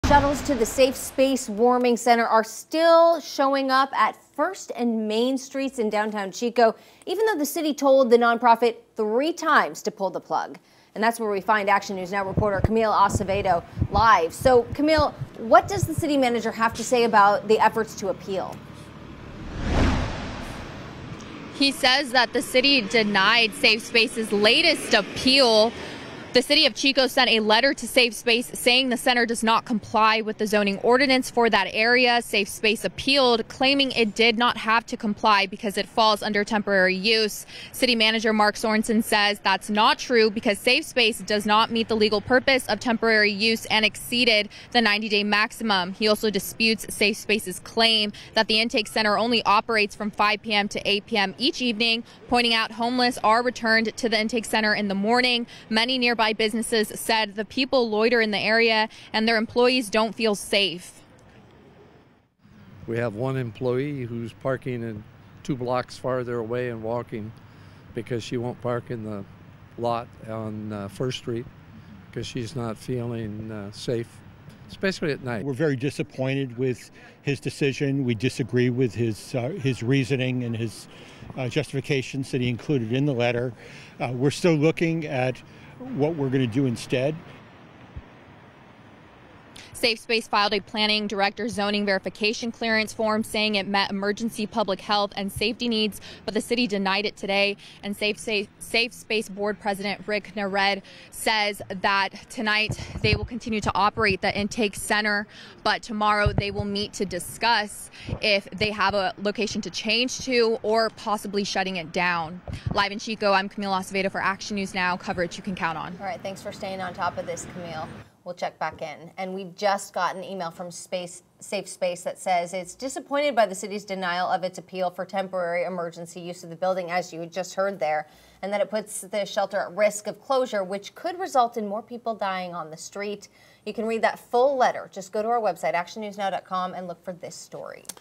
Shuttles to Safe Space Warming Center are still up at 1st and Main Street
Safe Space responded to Sorensen's letter, just look for this story and you can read exactly what Safe Space had to say.